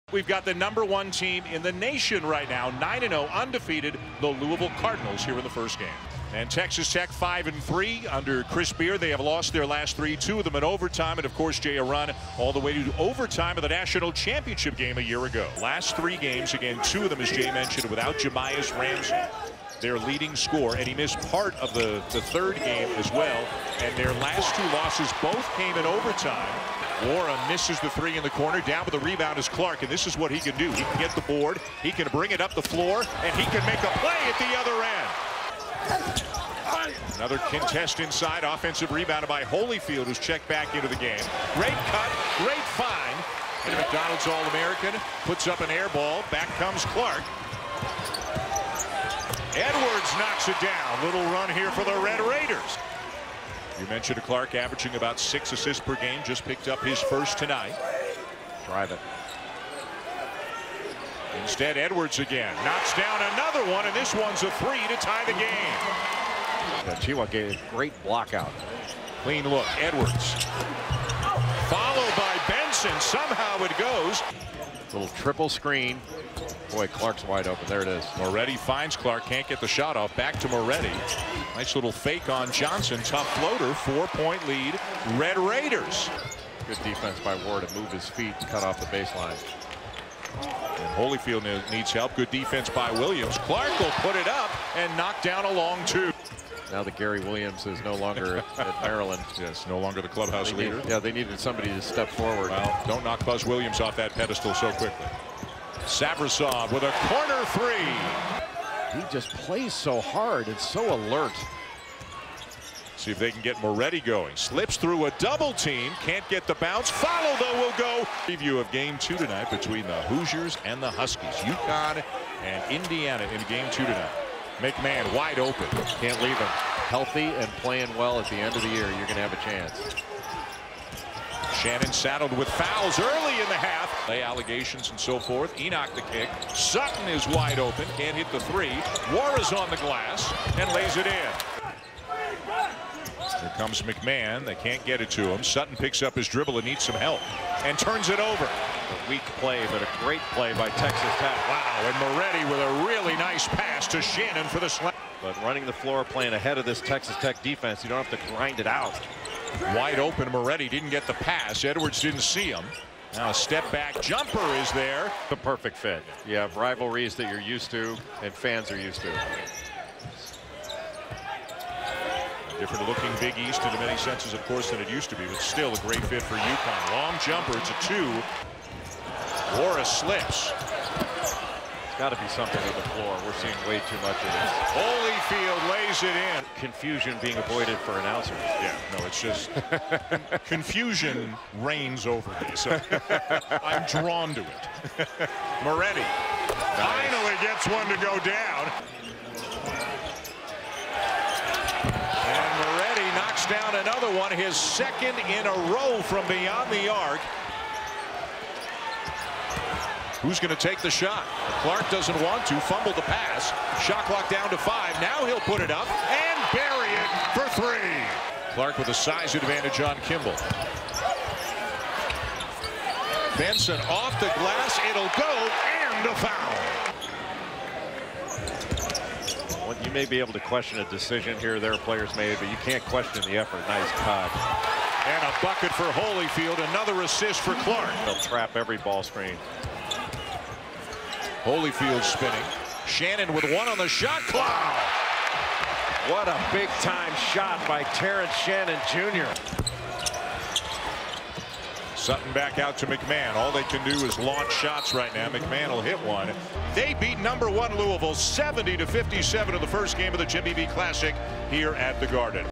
Texas Tech vs. Louisville Men's Basketball Highlights
Texas Tech upset No. 1 Louisvillle 70-57 in the Jimmy V Classic.